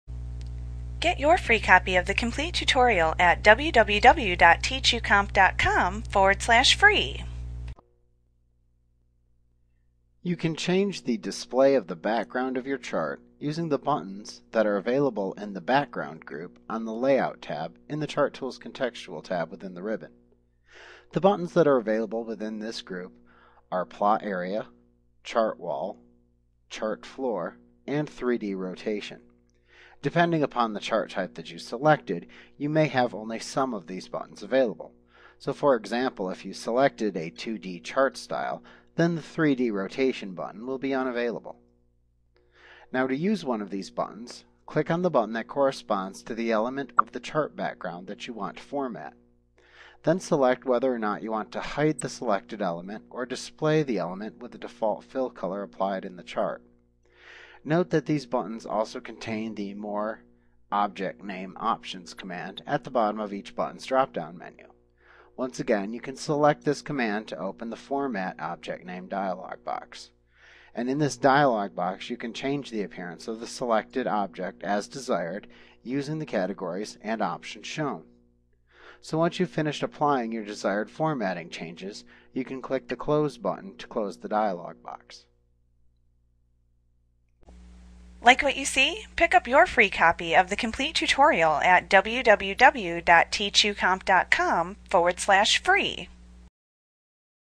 Excel 2010 Tutorial Chagning the Chart Background Microsoft Training Lesson 21.5
Learn how to change the chart background in Microsoft Excel A clip from Mastering Excel Made Easy v. 2010. - the most comprehensive Excel tutorial available. Visit us today!